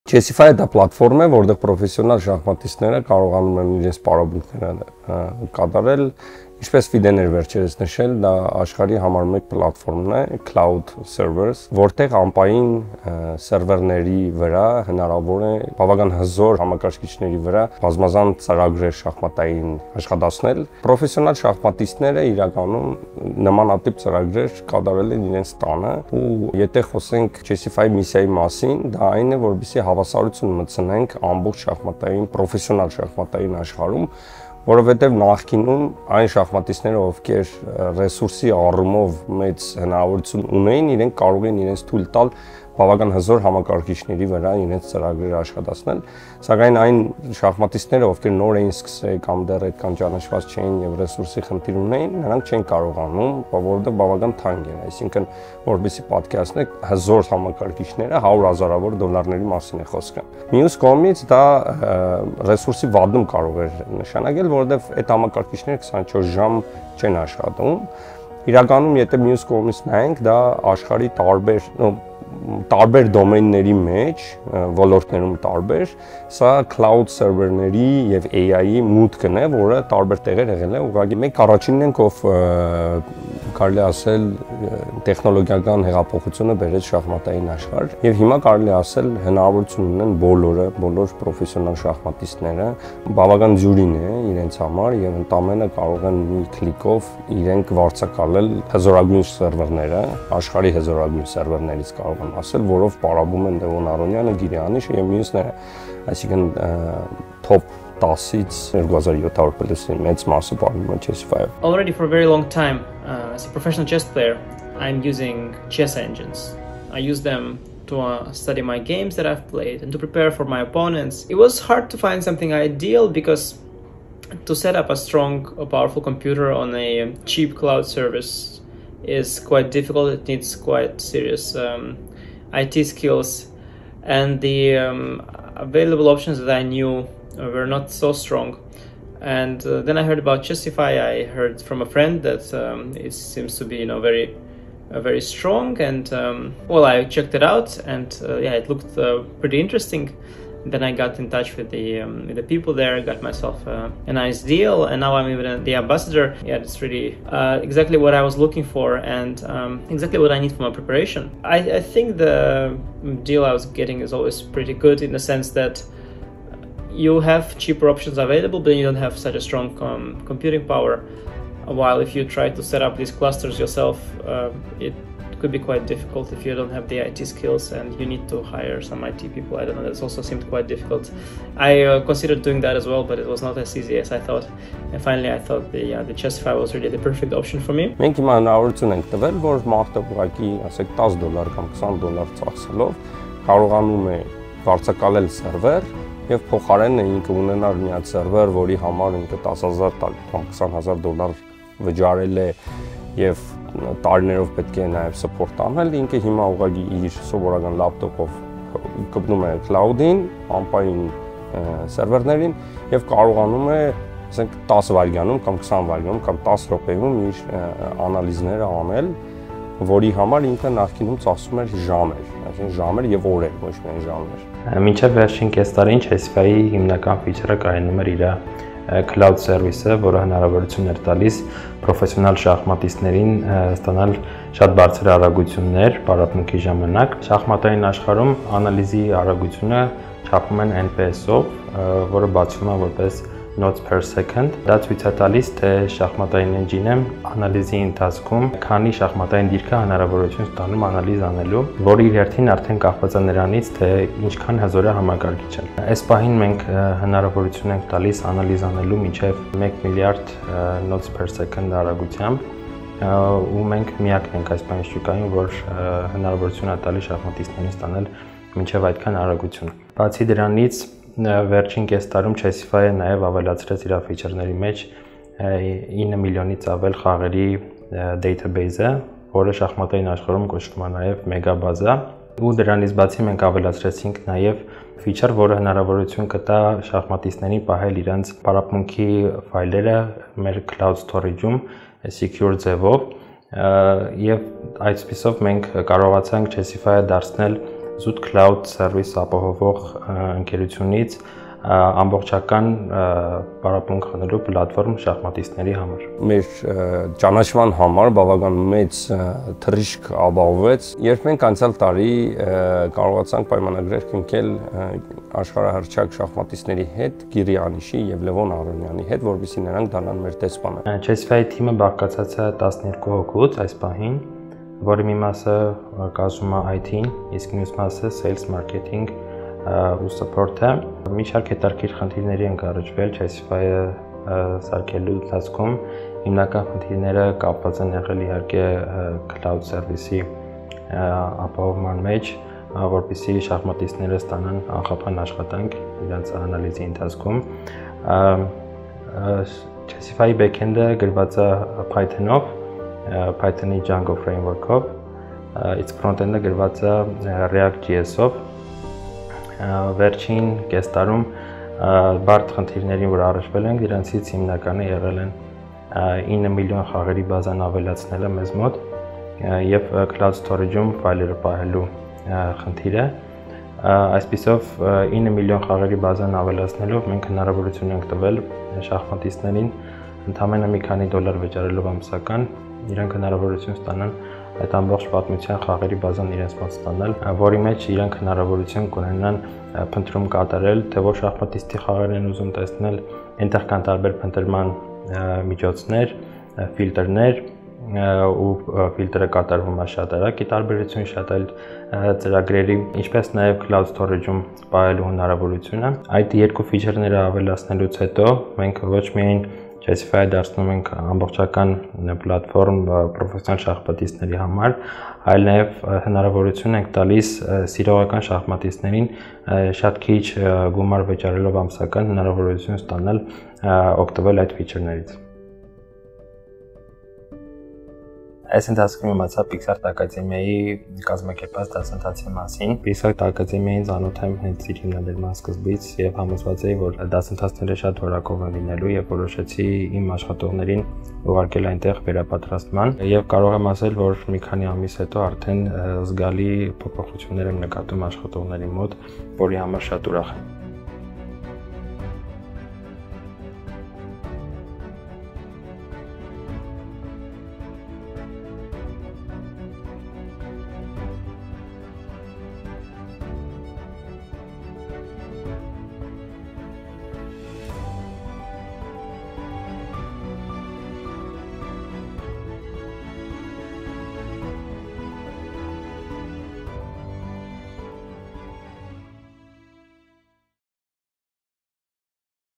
The Art of Chessify | Հայկական startup, որը խմբավորում է շախմատն ու տեխնոլոգիաները
Լևոն Արոնյան, Անիշ Գիրի և այլ գրոսմեյստերներ օգտագործում են հարթակը գրեթե ամեն օր, որտեղ շախմատն ու տեխնոլոգիաները ձուլվում են ստեղծագործական մտքի հետ։